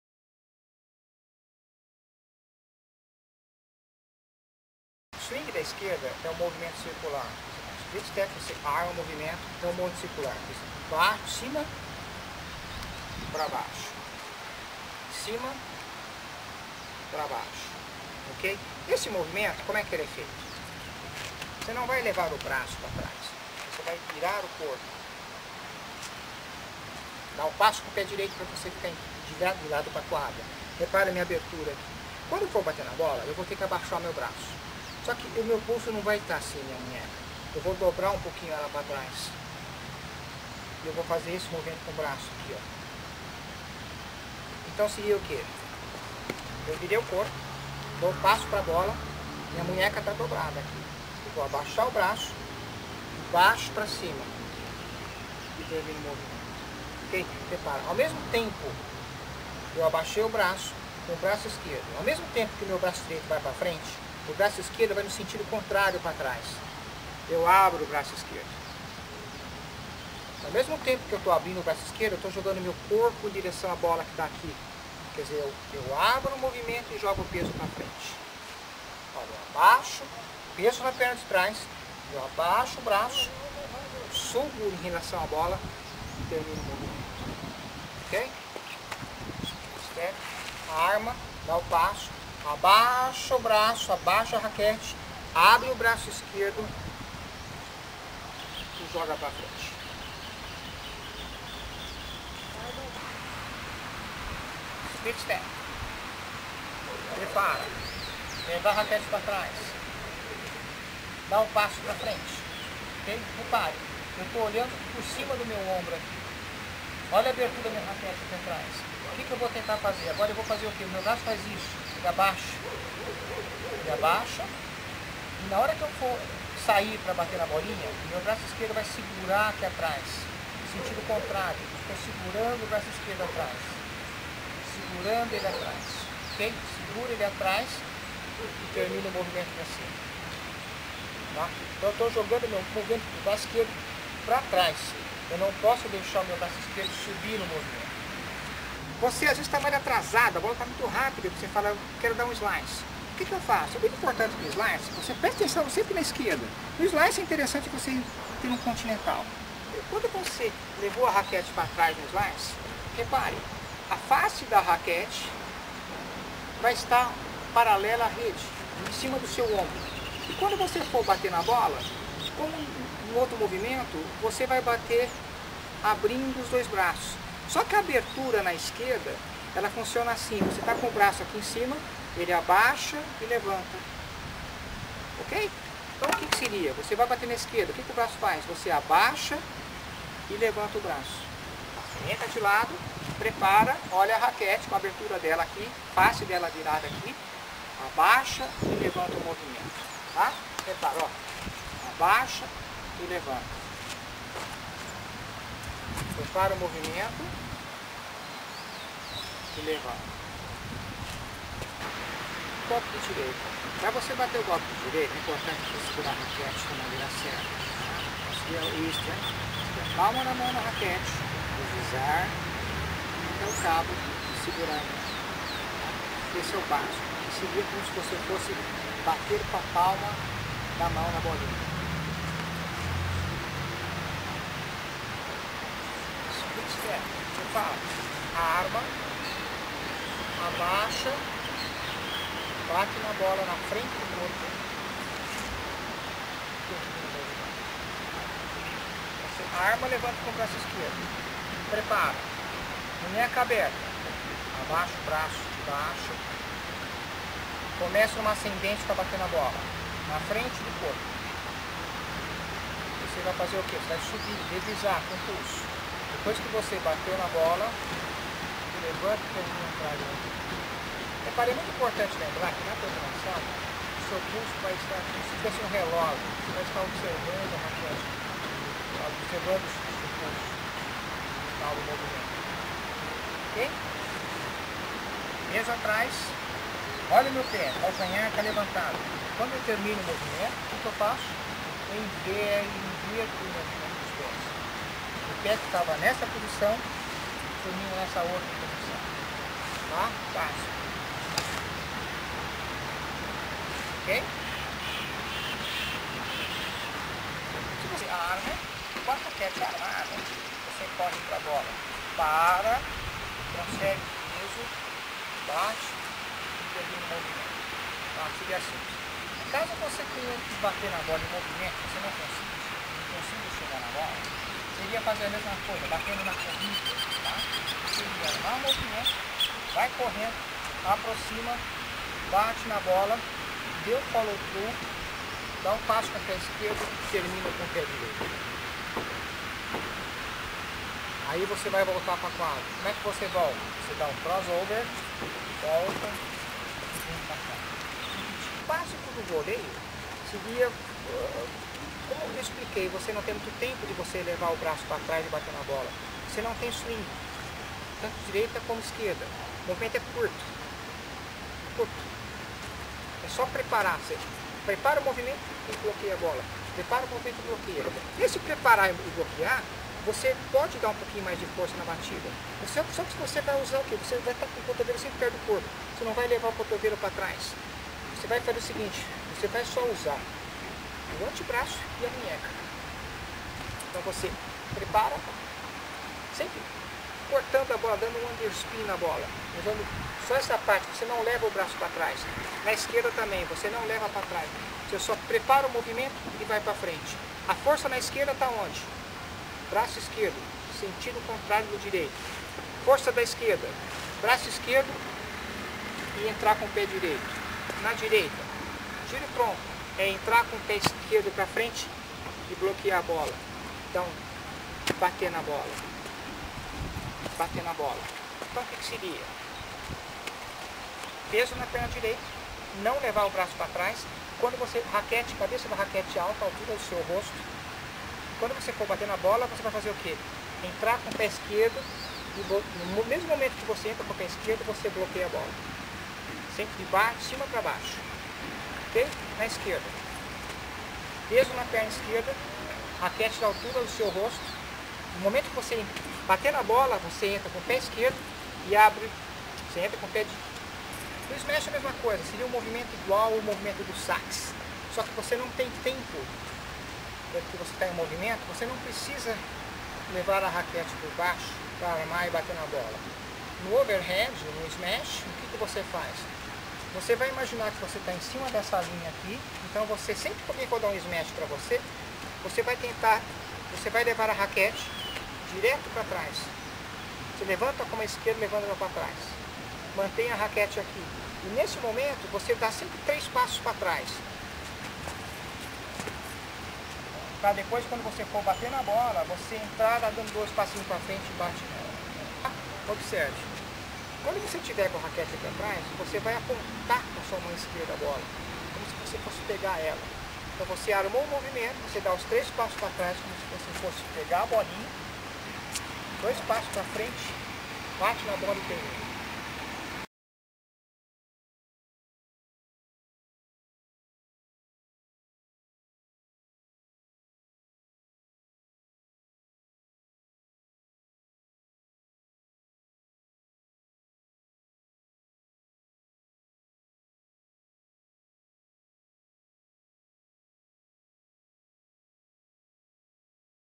Tênis Voleio Smash Direita Esquerda - Tenis Dicas para melhorar seu jogo
Dicas do Voleio  Smash Direita e Esquerda (Volley Over head forehand backhand) , professor Newton